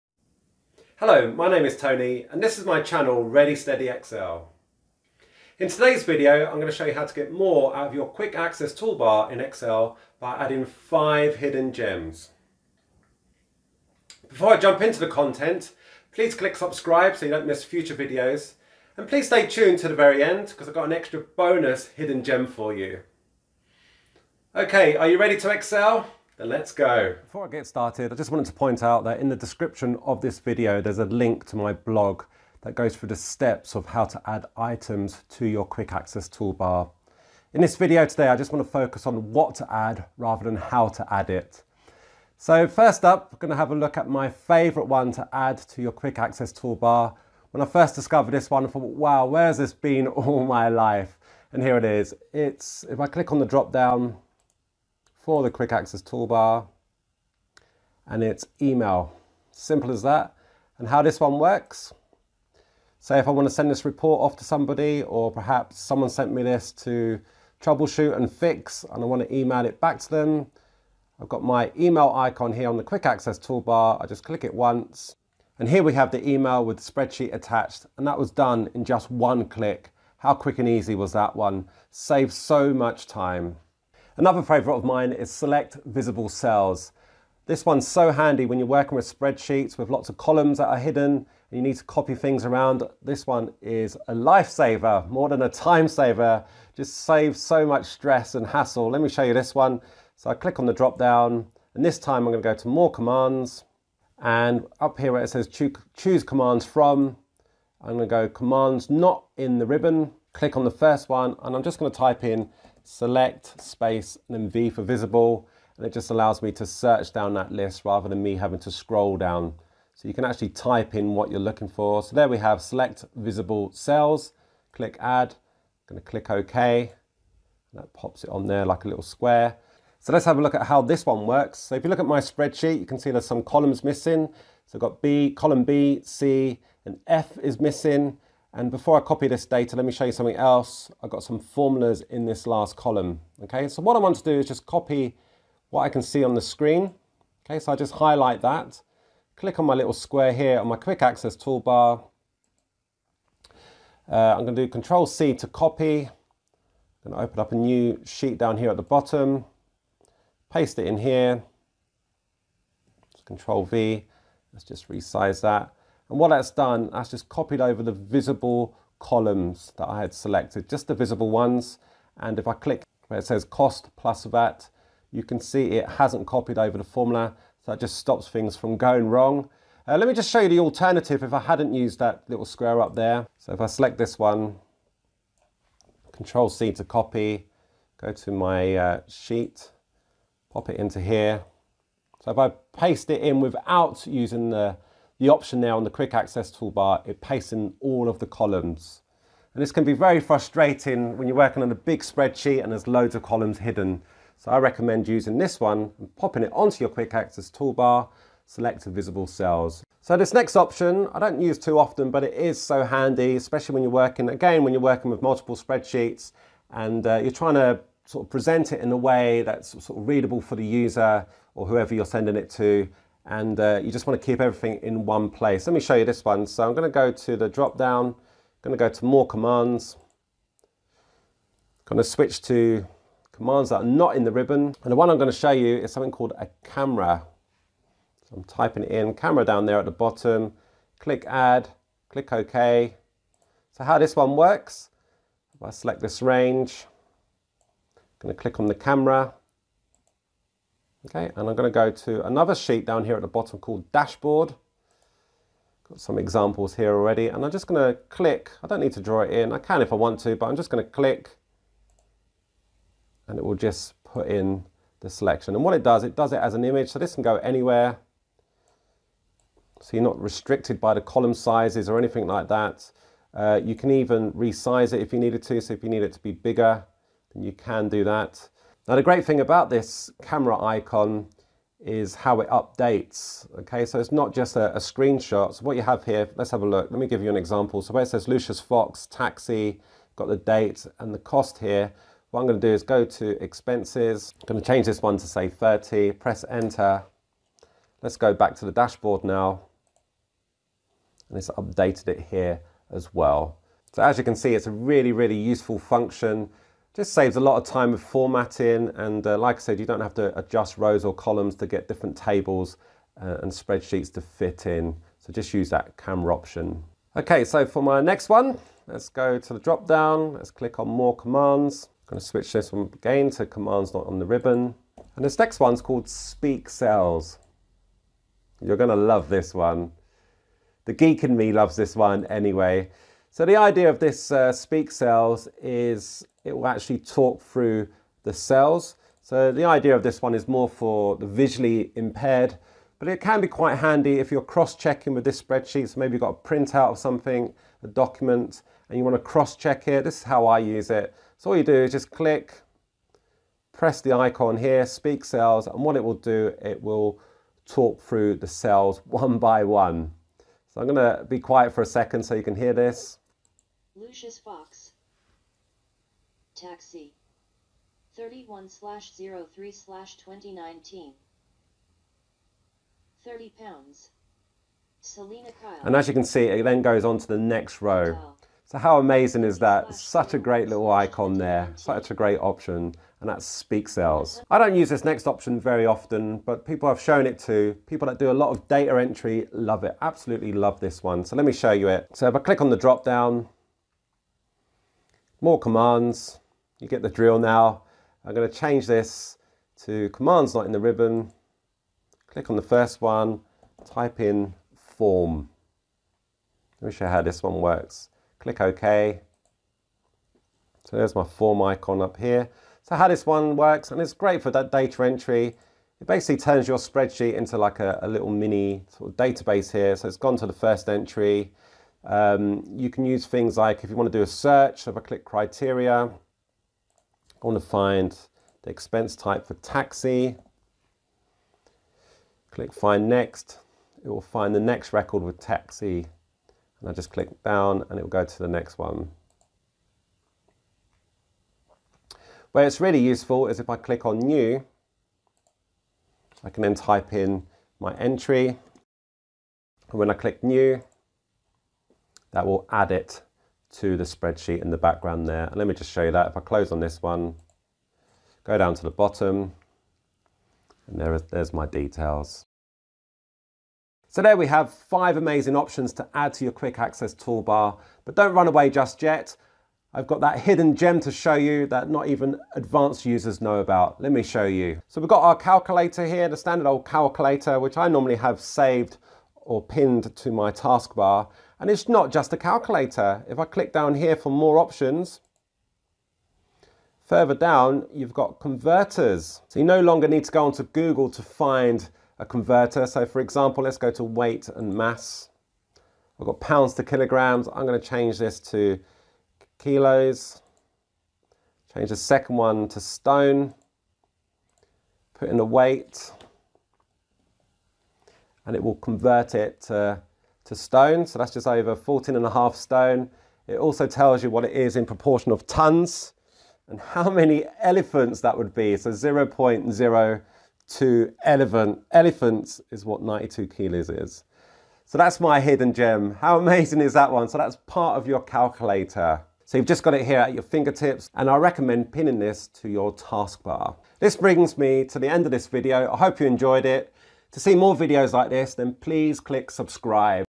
5 Hidden Gems to add to your Excel Toolbar
Learn how to customise the Quick Access Toolbar in Excel with these 5 hidden gems.

The video covers commands that are not available on the ribbon. Add these 5 options to your toolbar to speed productivity:

- Email spreadsheet from Excel
- Select Visable Cell (Great for copying tables with hidden cells)
- Camera (Take a live snapshot of your table)
- Speak Cell (Tool for the visually impaired but also great for cross-checking)
- Form (Makes data entry into Excel easier when adding large amounts of information)